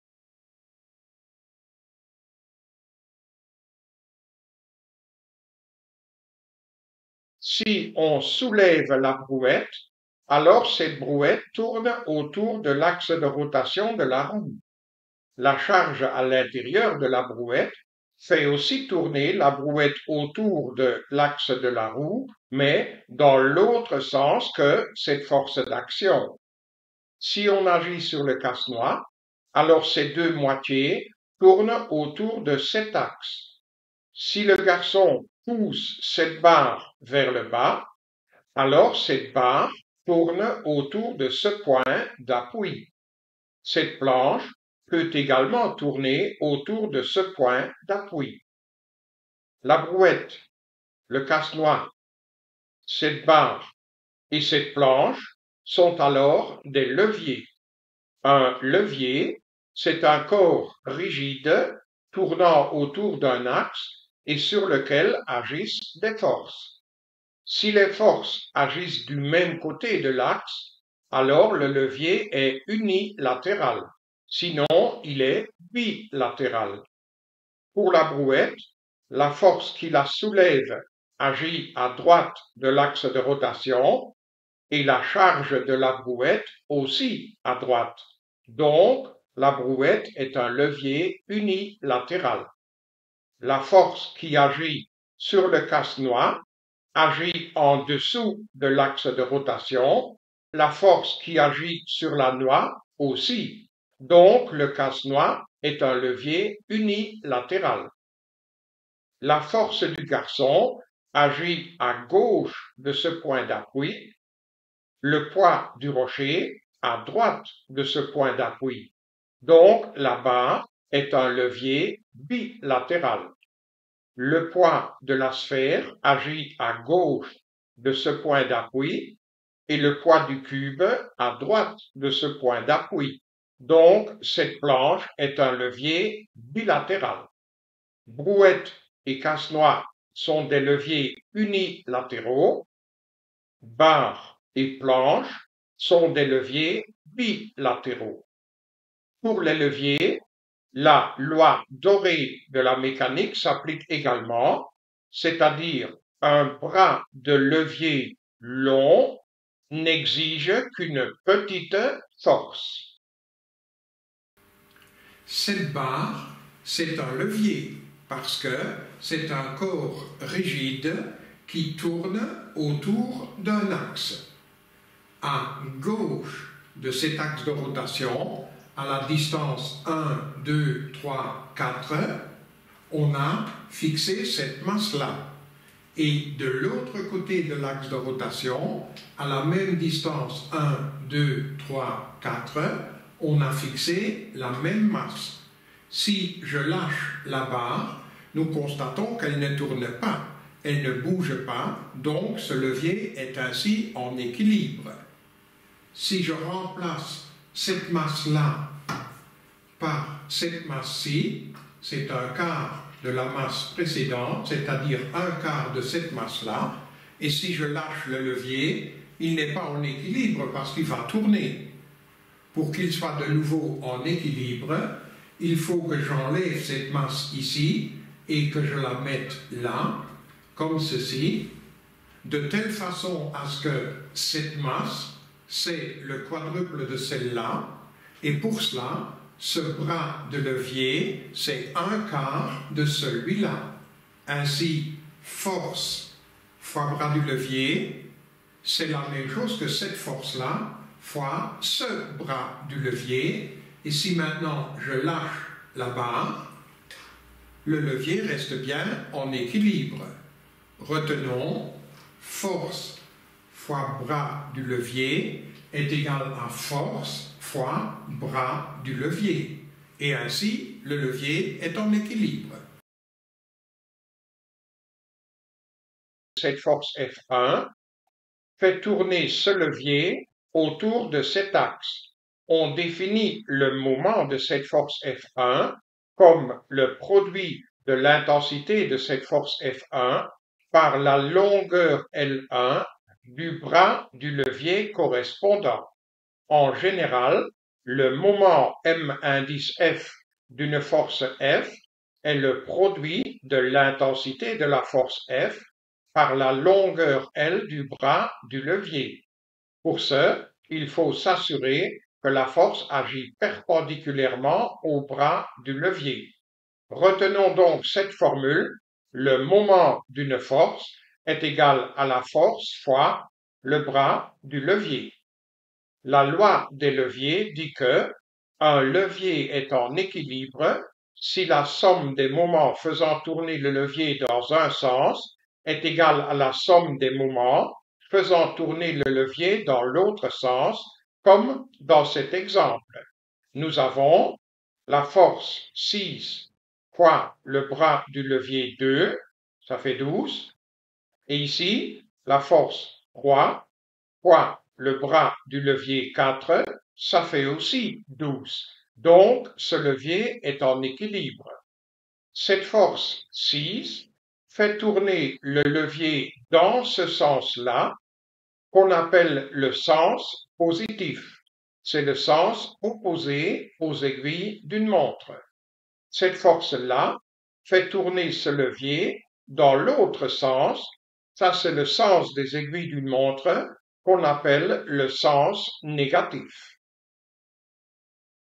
Physique 3PhF022 Leviers
Qu'appelle-t-on un levier, un levier unilatéral et un levier bilatéral? Quelle est la loi des leviers? Quand un levier est-il en équilibre? Qu'appelle-t-on le moment d'une force?